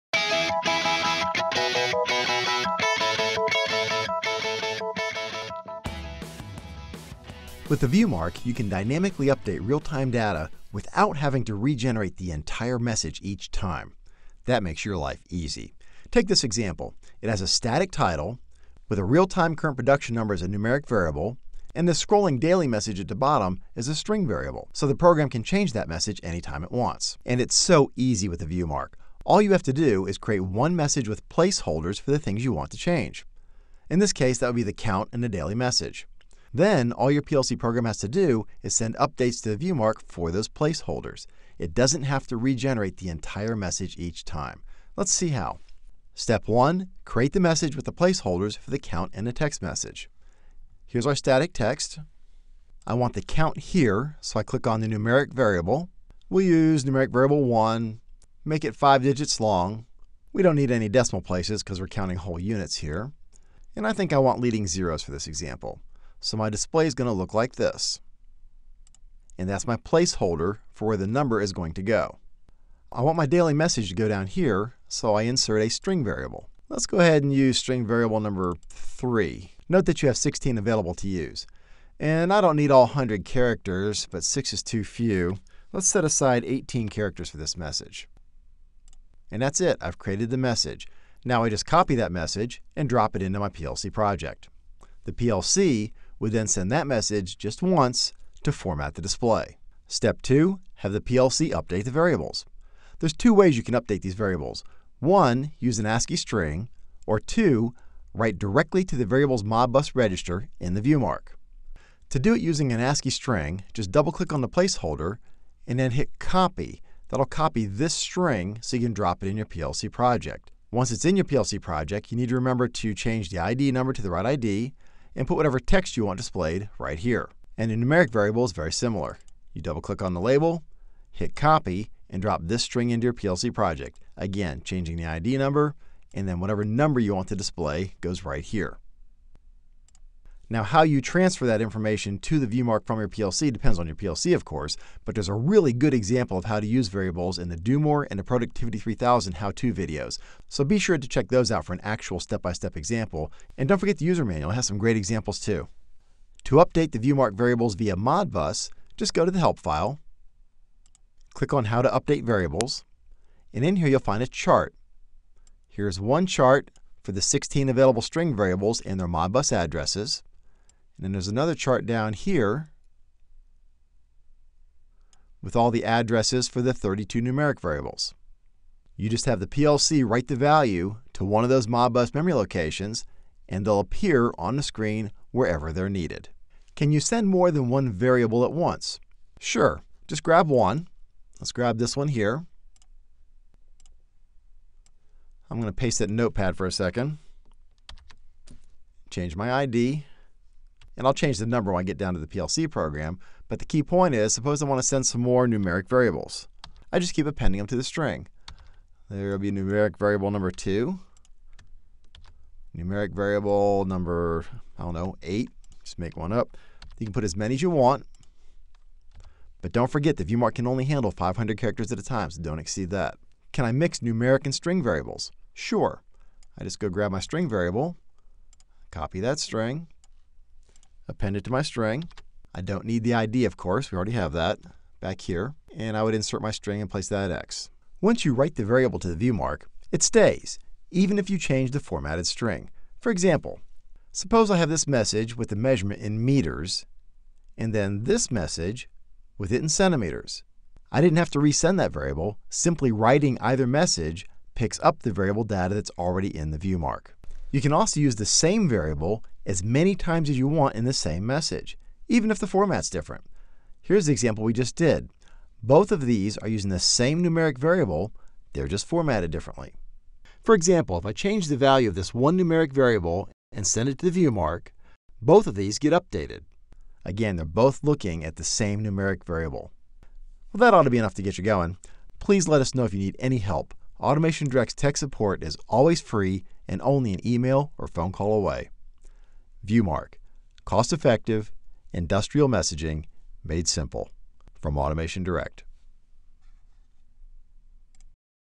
ViewMarq LED Message Display Variables from AutomationDirect
Learn how to use numeric and text variables to help make your PLC projects easier to use and maintain.